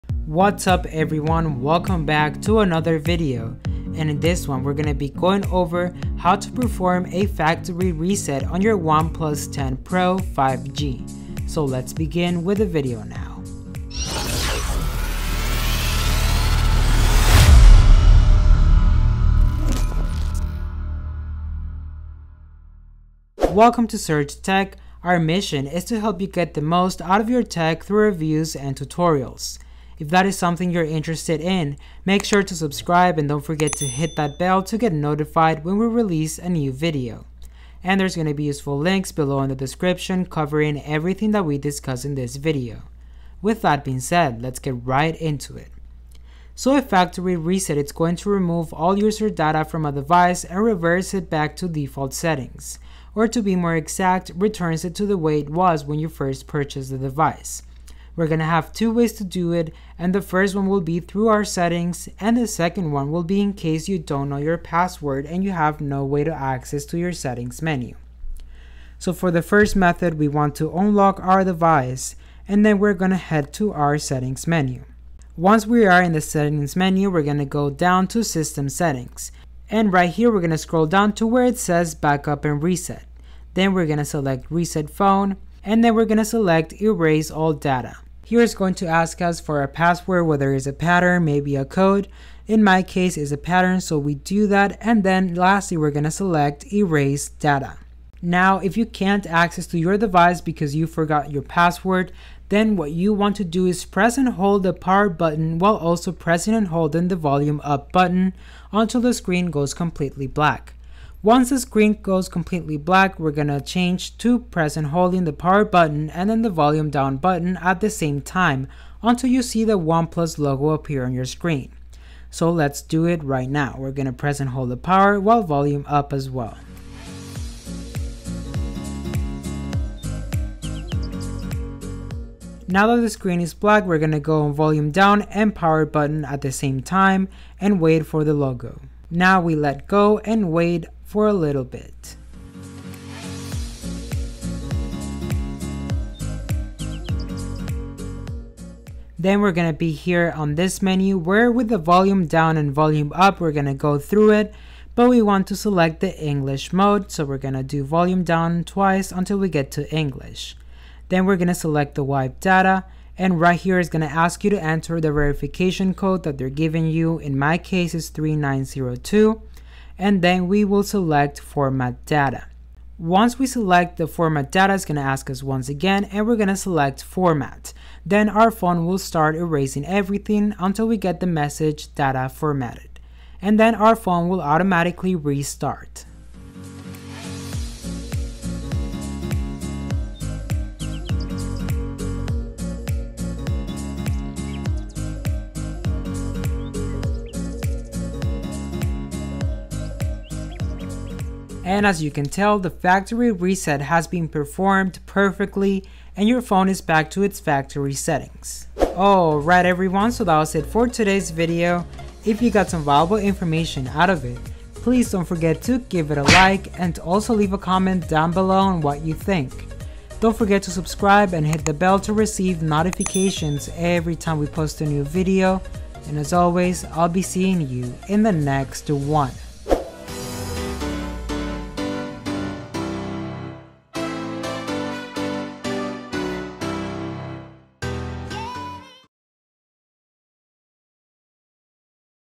How To Reset OnePlus 10 Pro 5G - Hard Reset
Did you forget your password? Or is your phone running very slow or freezing? Try a hard reset.

This is a video on how to perform both methods of factory reset on a OnePlus 10 Pro 5G. Also known as a hard reset.

PS - Works for other OnePlus phones